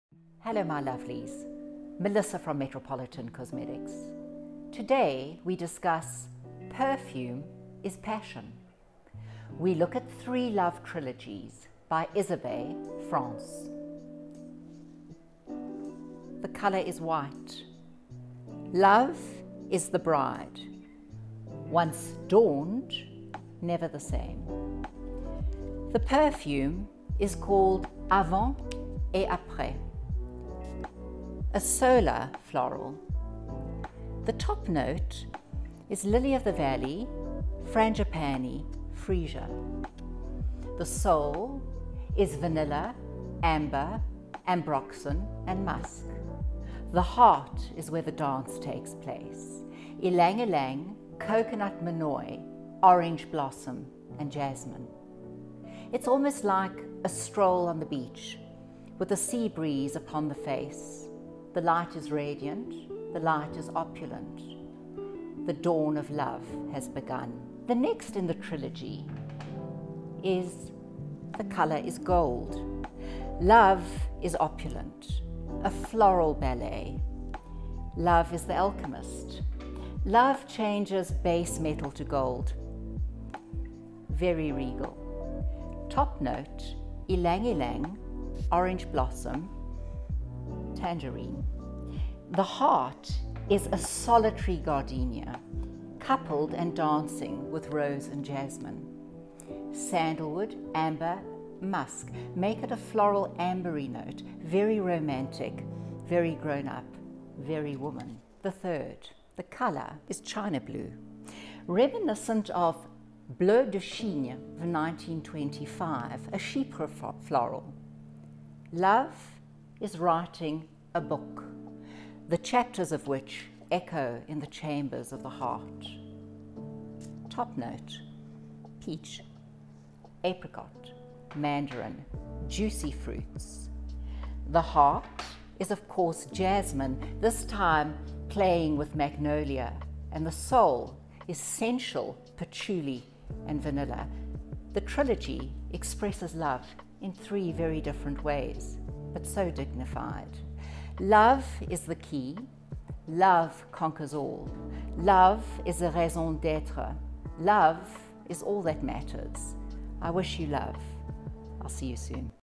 Express yourself with this trilogy from Isabey Paris
Perfume is Passion

Melissa explores the heart of three evocative love stories from the Iconic Isabey Paris France perfume collection.

The Colour is White
Avant et Apres by Isabey is a Floral Woody Musk perfume.

The Colour is Gold
Isabey Gardenia by Isabey is a Amber Floral perfume.

The colour is China Blue
Isabey Fleur Nocturne by Isabey is a Chypre Floral perfume.